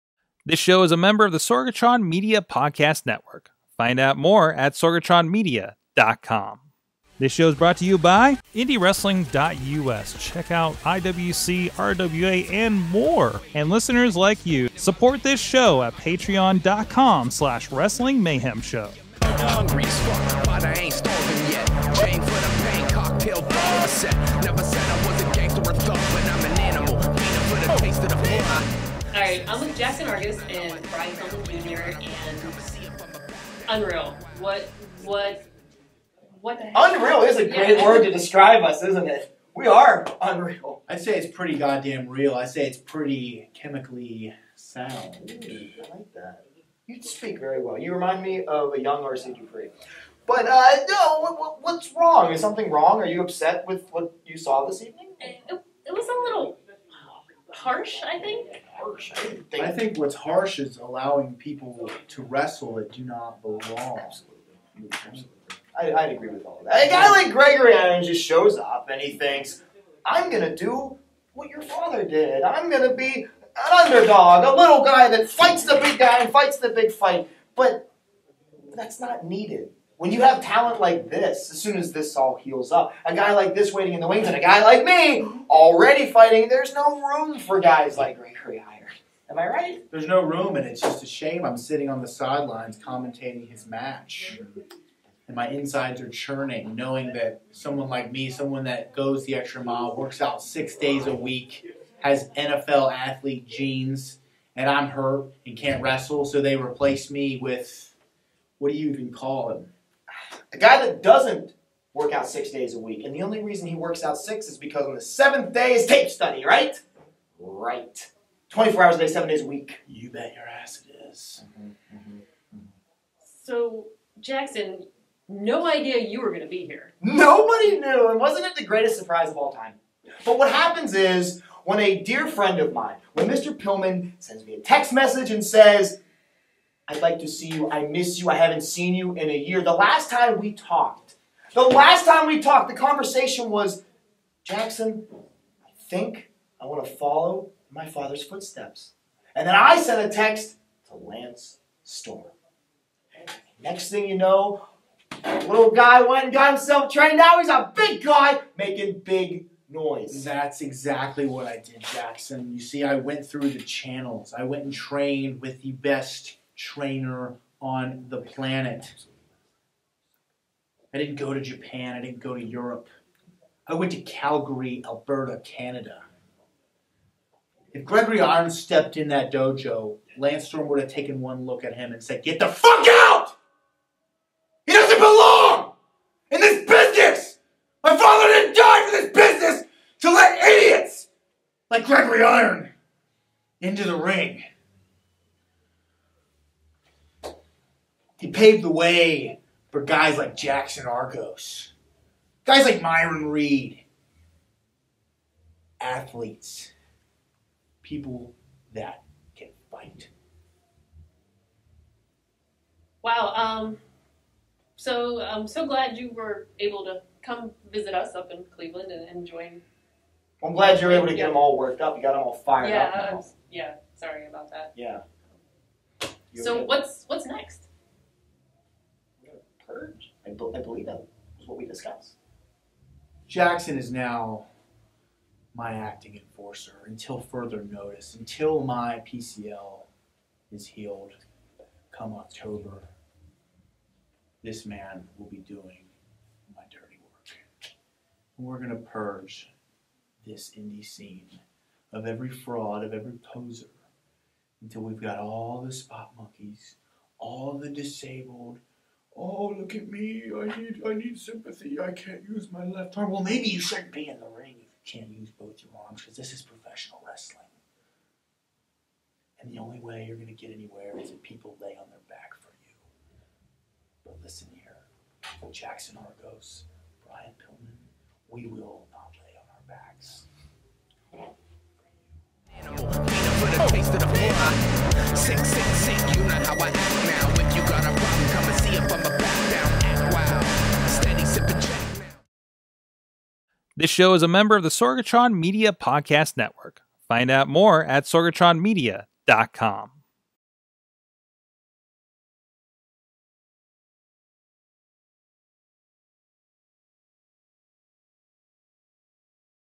Indy Mayhem Show On Location: Jaxon Argos & Brian Pillman Jr.
We talk with Jaxon Argos and Brian Pillman Jr shortly after their confrontation with Gregory Iron at Welterweight Wrestling 3.  Brian talks about his motivations and what’s next for the Lance Storm School Graduates.

All interviews from back stage at Welterweight Wrestling 3.  and pick up Digital Downloads of past events on IndyWrestling.us!